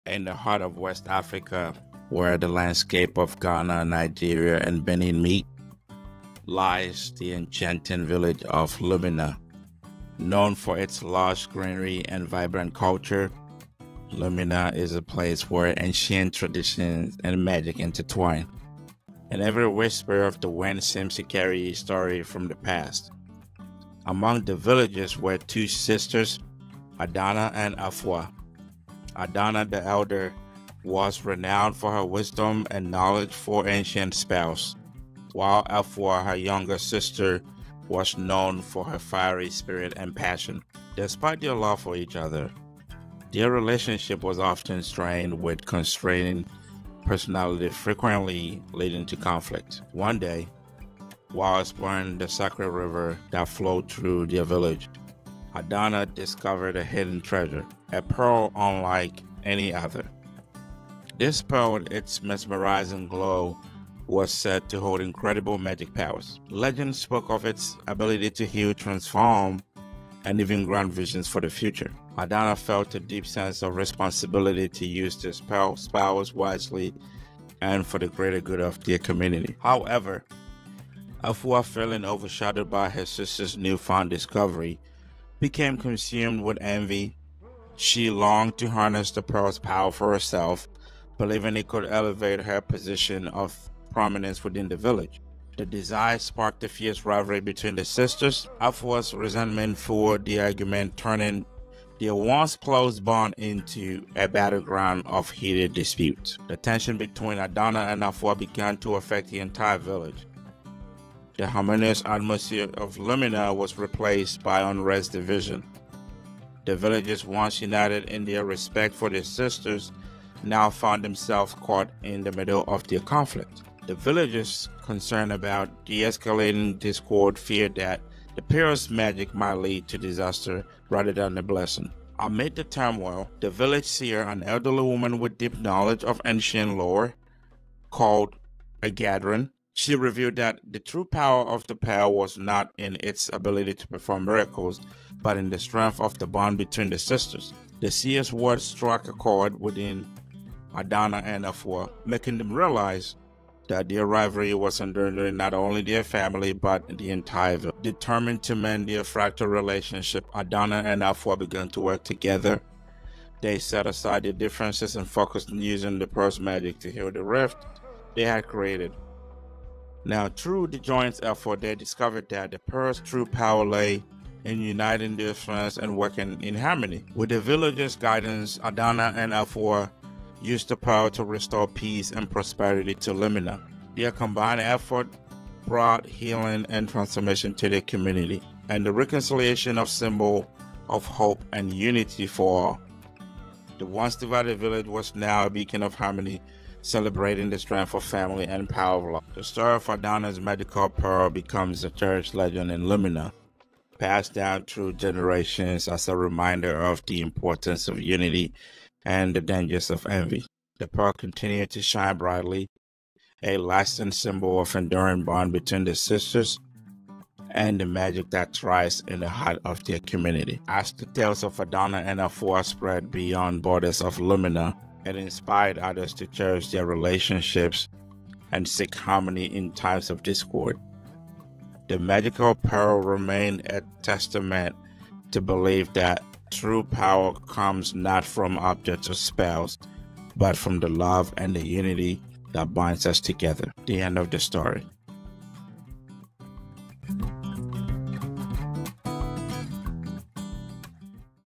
SHE WAS JEALOUS OF HERE OWN SISTER!
Addana's Magical Pearl: The Epic Tale of Sisterly Rivalry and Redemption 🌟

Step into the heart of West Africa with our latest episode, "Addana's Magical Pearl," where ancient legends come to life! 🏞️✨

In the vibrant village of Lumina, two sisters, Addana and Afua, are torn apart by jealousy and rivalry over a mystical pearl with incredible powers. 🌺🔮 This magical artifact, discovered by the wise Addana, is said to heal, transform, and reveal visions of the future. But when Afua's envy spirals out of control, it sets off a series of events that threatens to destroy their family and their entire village.

Watch as the tension between the sisters escalates, leading to a dramatic family dispute and a powerful journey of reconciliation. Discover how the true magic of the pearl lies not in its power, but in the strength of their bond and the unity it can restore. 🌈💖

Join us on Africa Chat for a captivating animation filled with West African culture, legendary folklore, and the timeless lesson that love and unity can overcome even the greatest of conflicts.